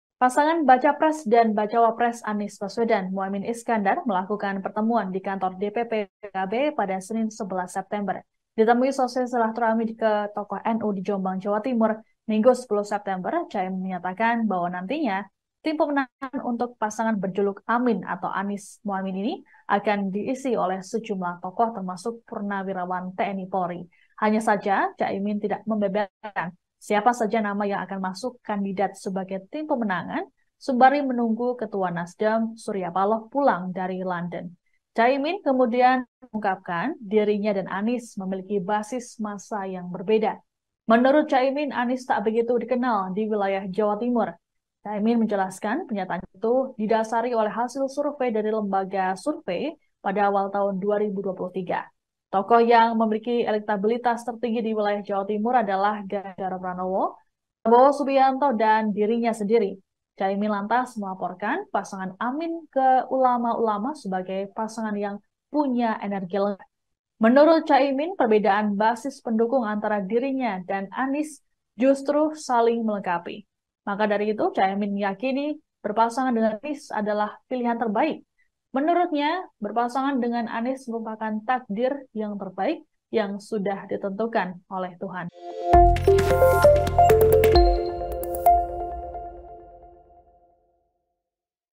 Cak Imin Bakal Libatkan Purnawirawan TNI-Polri Untuk Pemenangan Anies Baswedan
Pasangan bacapres dan bacawapres Anies Baswedan dan Muhaimin Iskandar melakukan pertemuan di Kantor DPP PKB pada Senin (11/9/2023).

Ditemui seusai silahturahmi ke tokoh NU di Jombang, Jawa Timur, Minggu (10/9/2023), Cak Imin menyatakan bahwa nantinya tim pemenangan untuk pasangan berjuluk AMIN (Anies-Muhaimin) ini akan diisi oleh sejumlah tokoh termasuk purnawirawan TNI-Polri.

Hanya saja, Cak Imin tidak membeberkan siapa saja nama yang akan masuk kandidat sebagai tim pemenangan, sembari menunggu Ketua NasDem Surya Paloh pulang dari London.

Cak Imin kemudian mengungkapkan, dirinya dan Anies memiliki basis massa yang berbeda.

Menurut Cak Imin, Anies tak begitu dikenal di wilayah Jawa Timur.

Cak Imin menjelaskan pernyataannya itu didasari oleh hasil survei dari lembaga survei pada awal tahun 2023.